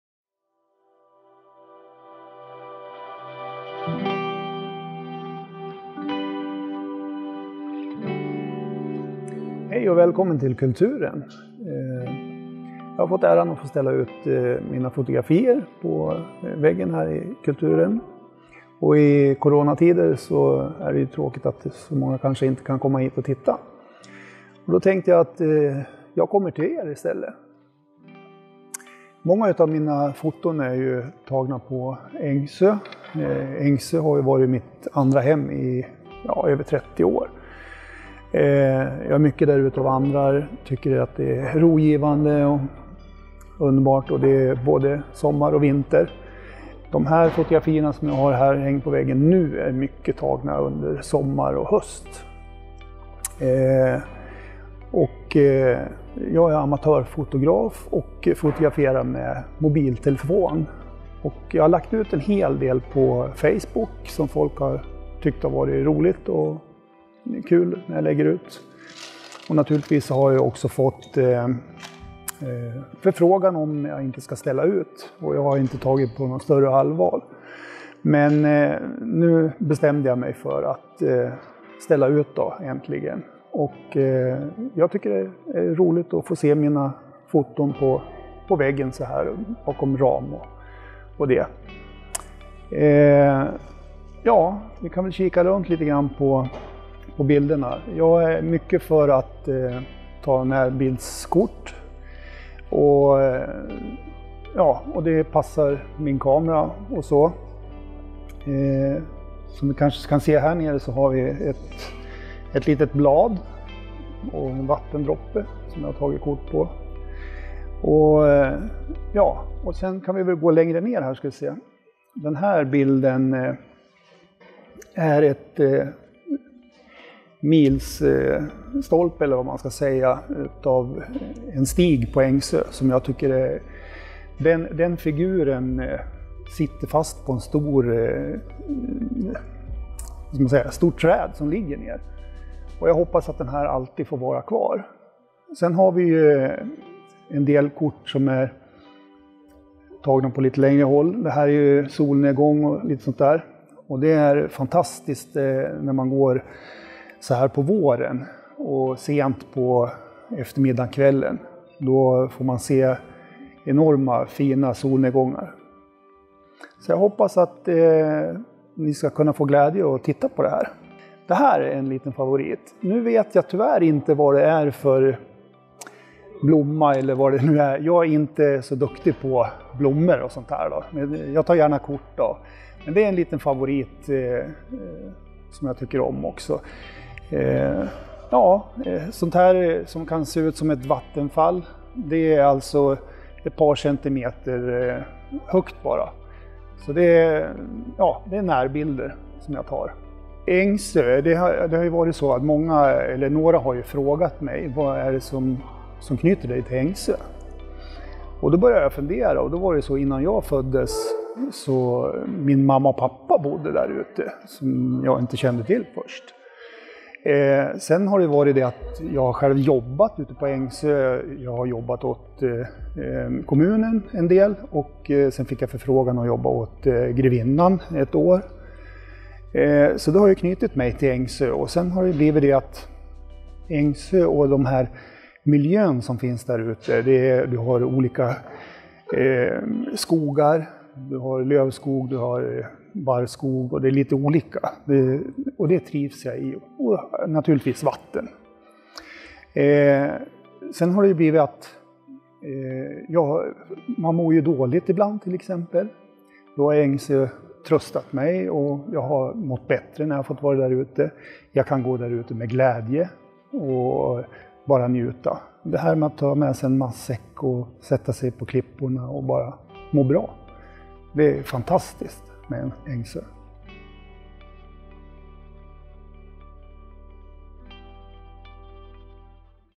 Ulfs digitala vernissage på CuLTUREN
Digital vernissage av Ulf Jönsson på CuLTUREN.
Fotoutställning av Ulf Jönsson 25 mars-20 april.
Naturen har alltid varit en stor del av mitt liv. Att få följa årstiderna på nära håll är helt fantastiskt. Ängsö har varit som mitt andra hem i ca 30 år och det är en plats där jag finner ro och laddar ny energi. Bilderna i utställningen är ett axplock från mitt älskade Ängsö